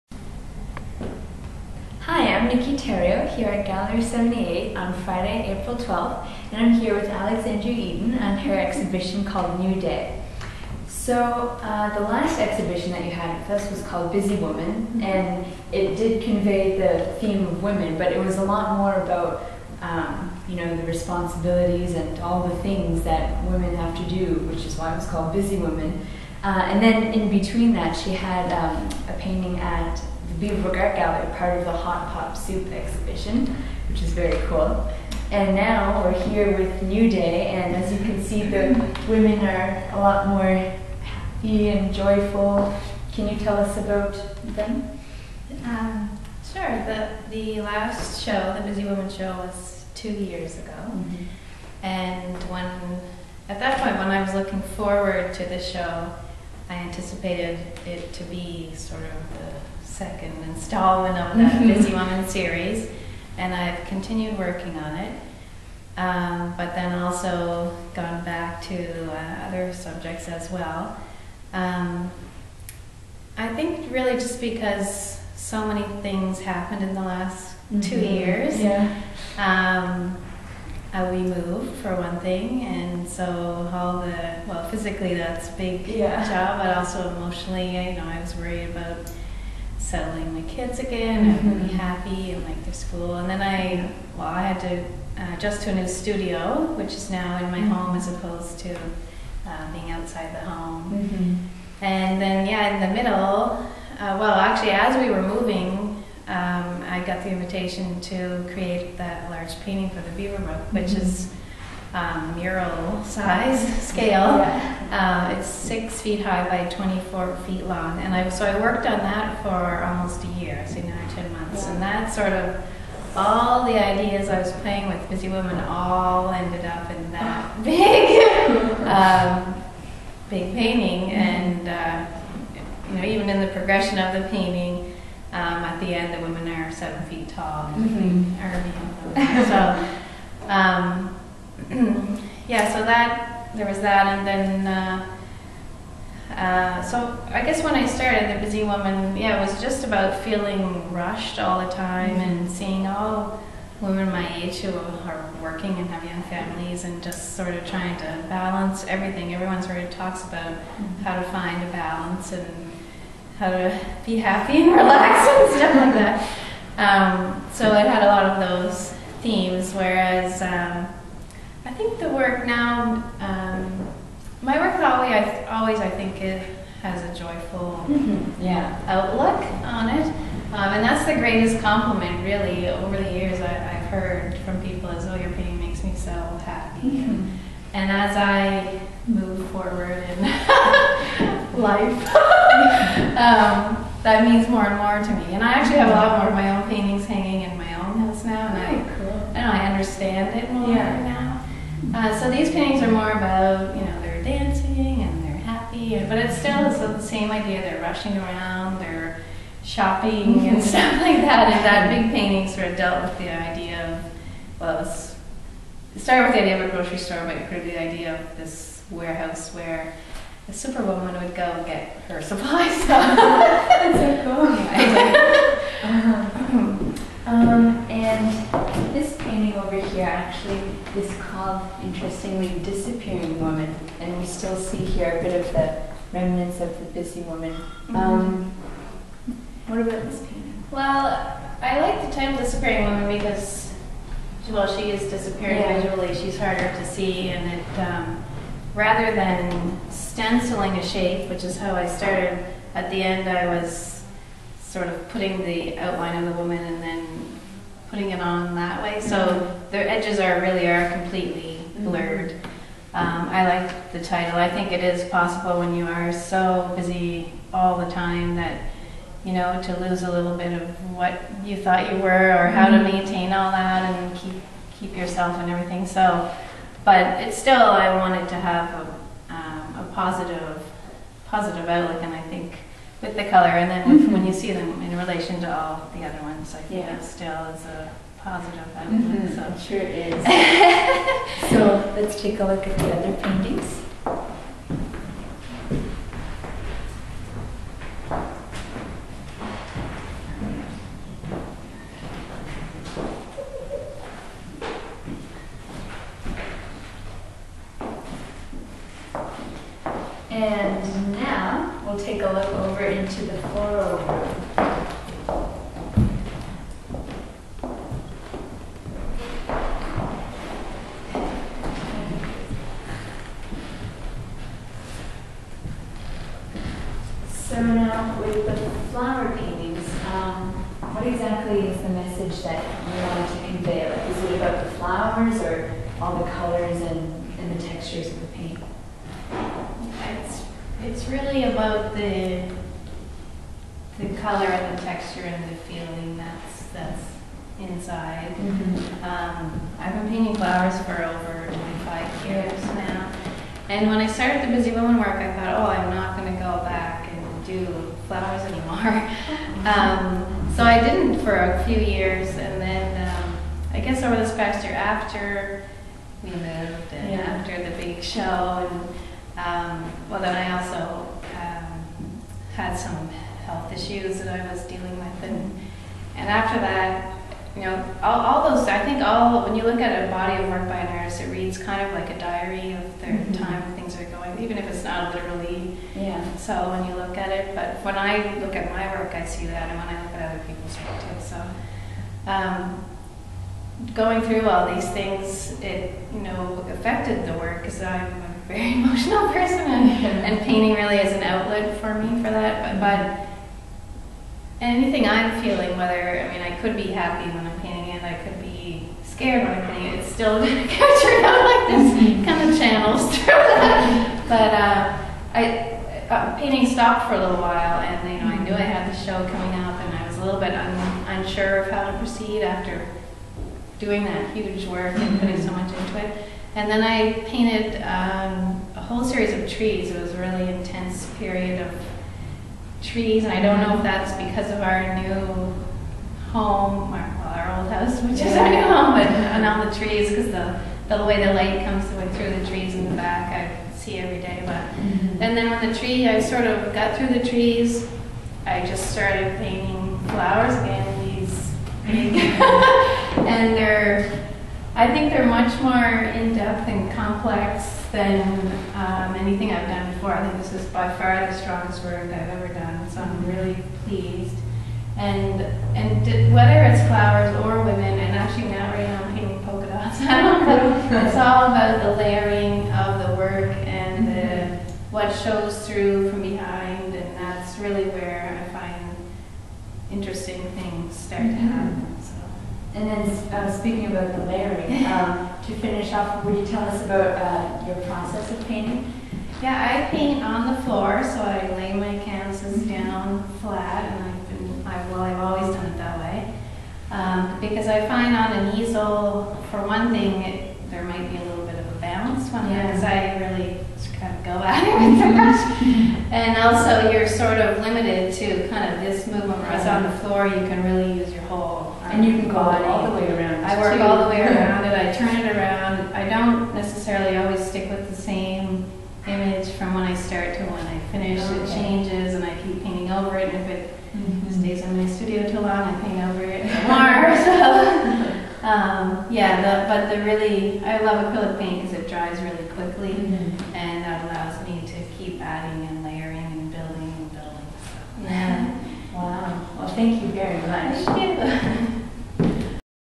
Alexandrya Eaton - New Day
Before her opening at Gallery 78 in Fredericton, on April 12, 2013 I took a few minutes to speak with Drya about the meaning behind her joyful woman-themed paintings, and her floral paintings, and of course about how she paints - which sounds like lots of fun!